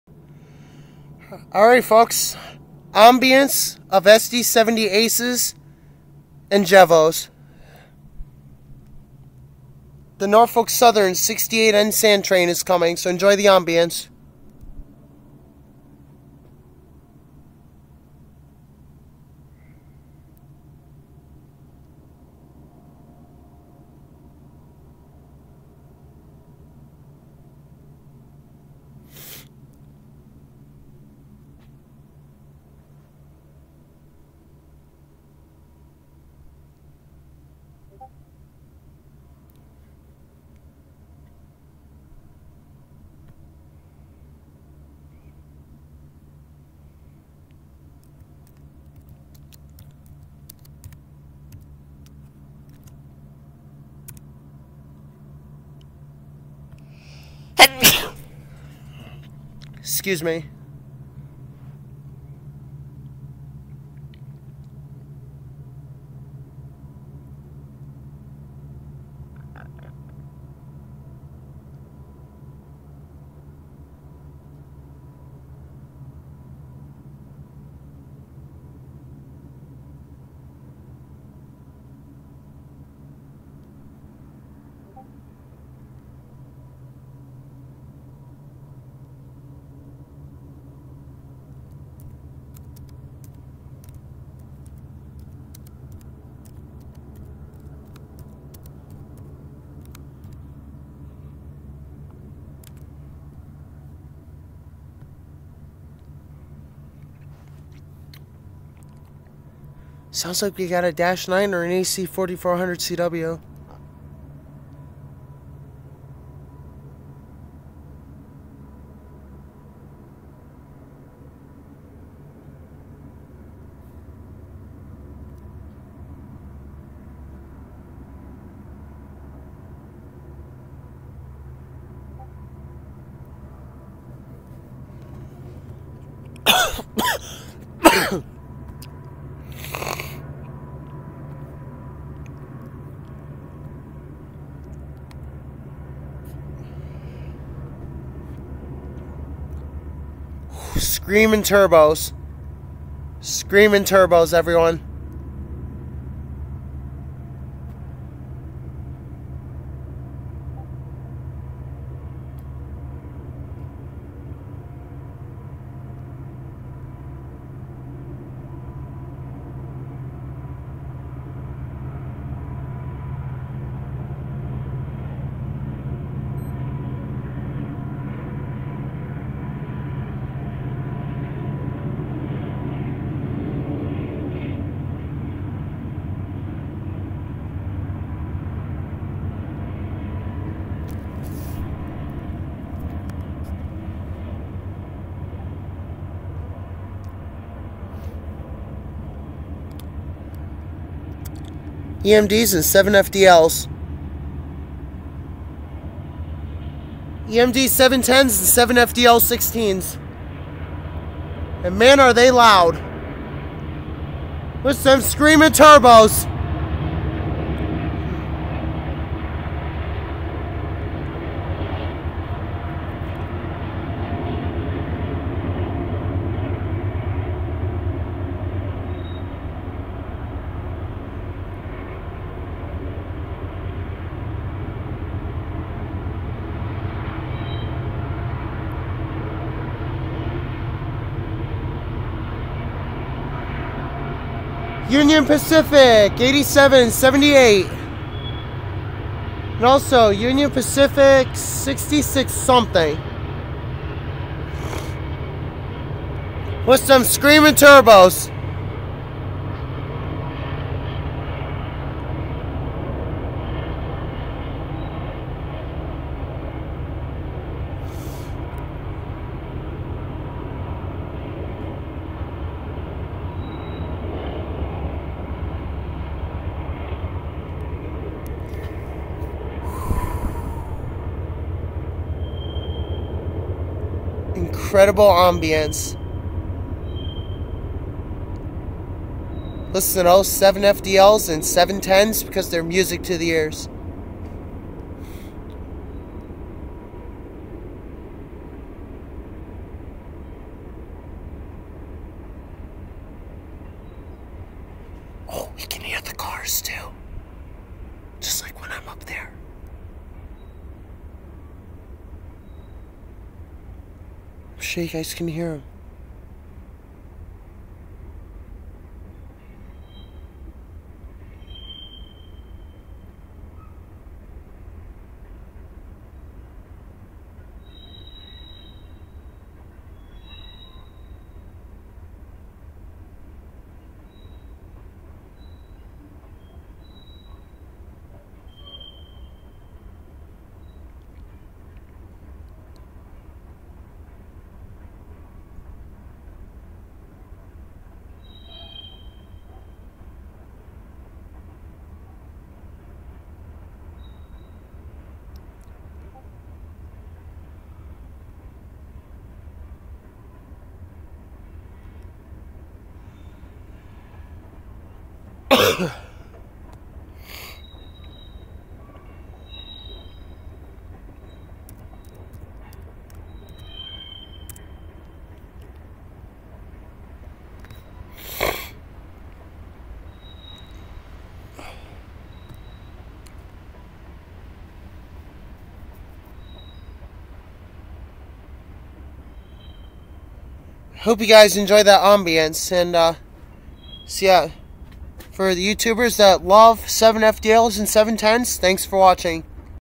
INCREDIBLE AMBIANCE!! 7FDL-16 and EMD 16-710 AT FULL POWER, SCREAMING TURBOS!!
Incredible sound!